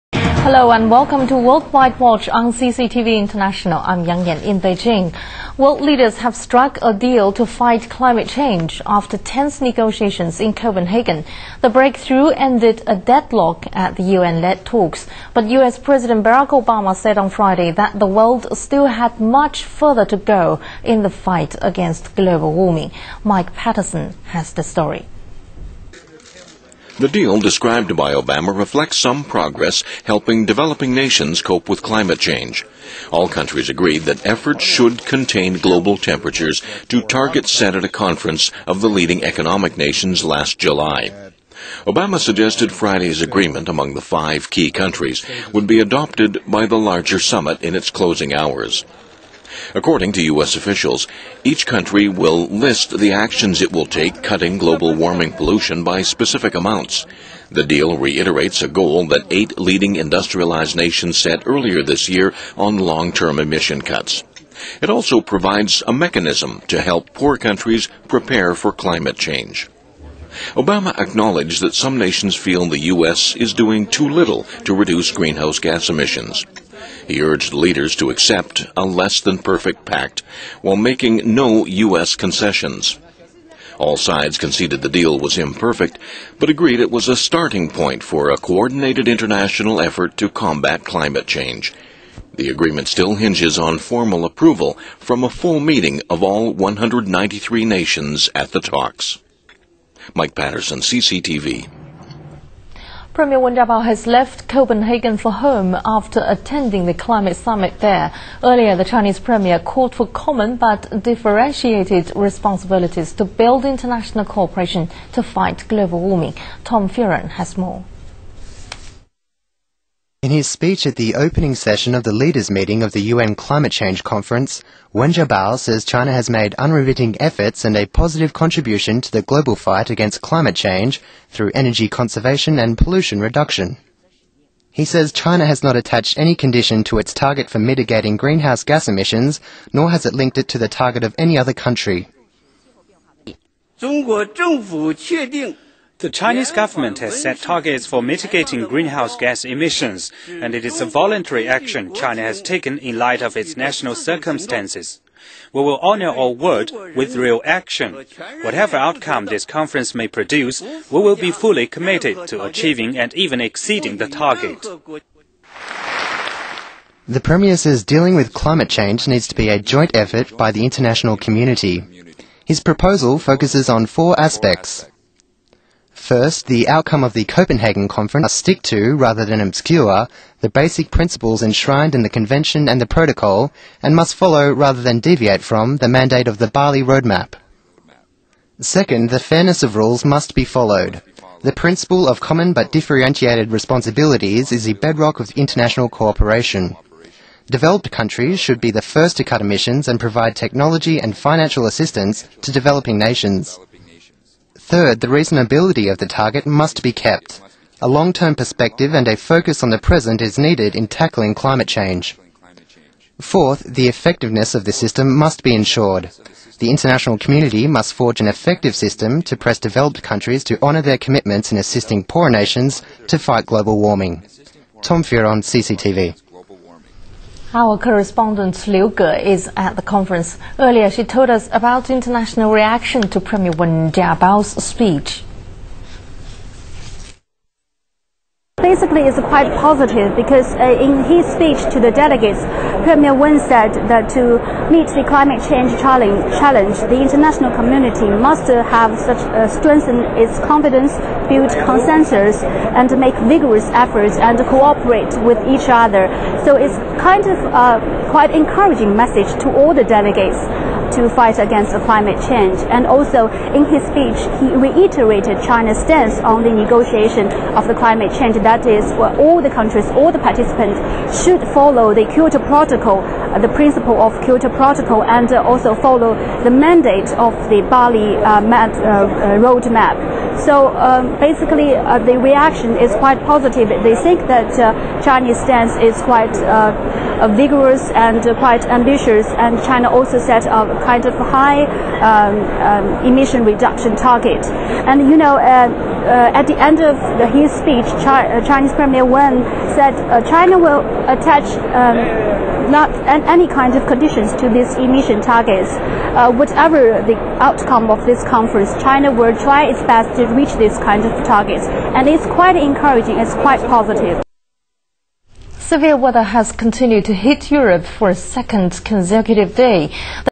Obama's "meaningful and unprecedented breakthrough" ends in deadlock - CCTV 091219
CCTV-9